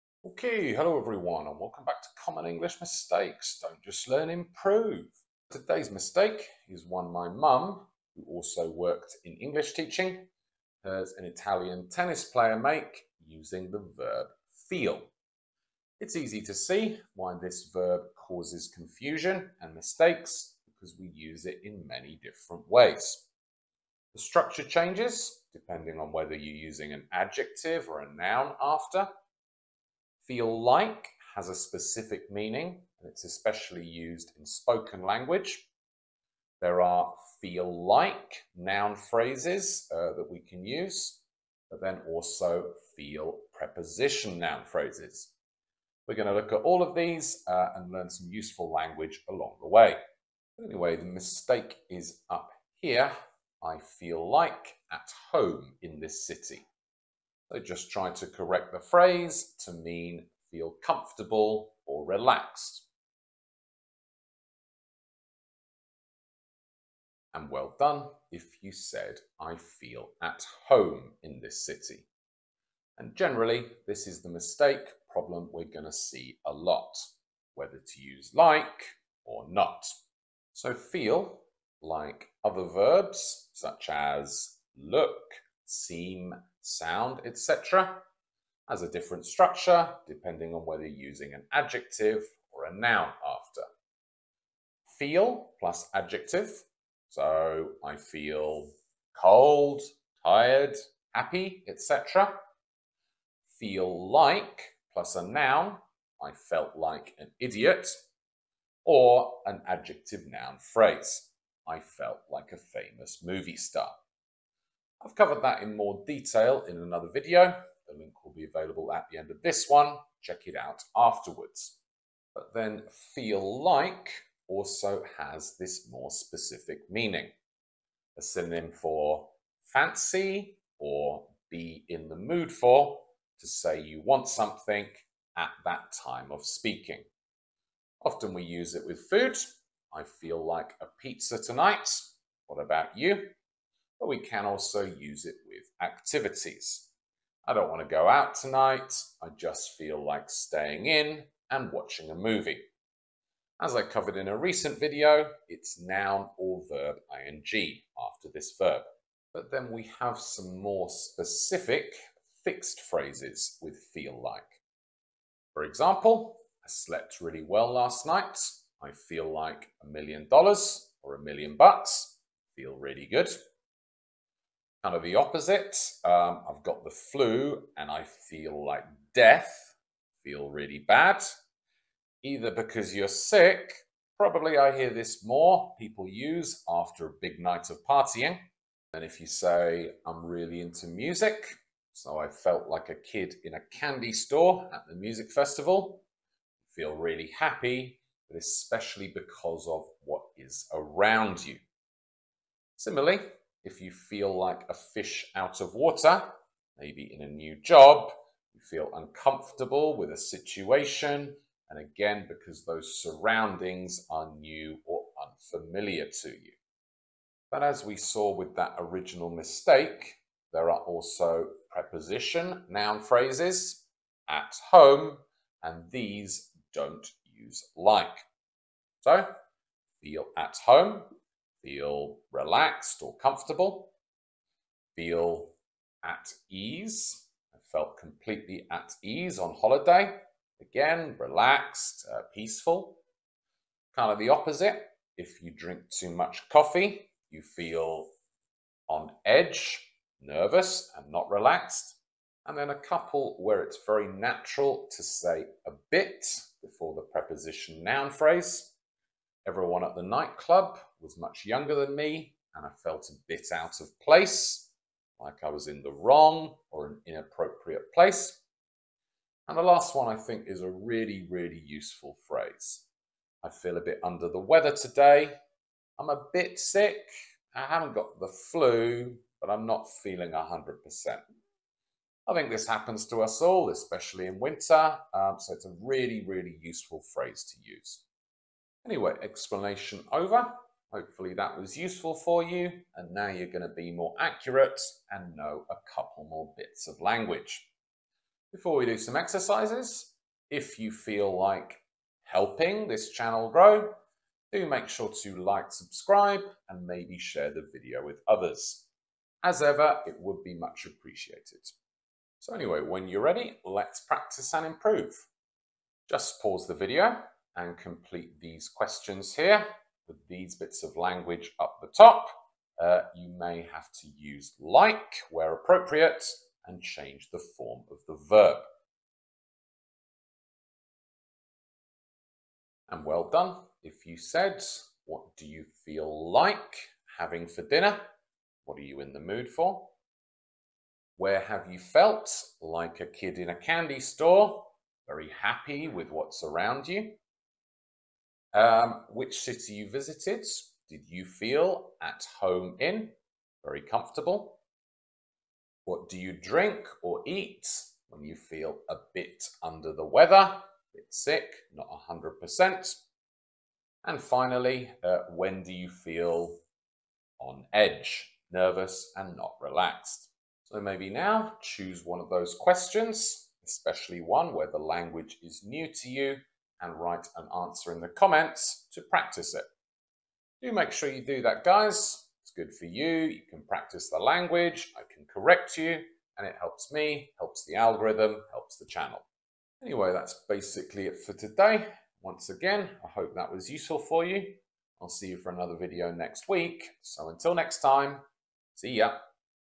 English ‘feel’ verb/phrases Mistake - “I feel like at home in this city.”
Hi everyone,

Today we’re going to look at few different ways we use the verb ‘feel’ sometimes with like and sometimes not.

This can be a little confusing, but after the video, you will have a better understanding of how to use it as well as some useful phrases to sound more natural.

Don’t just learn, improve!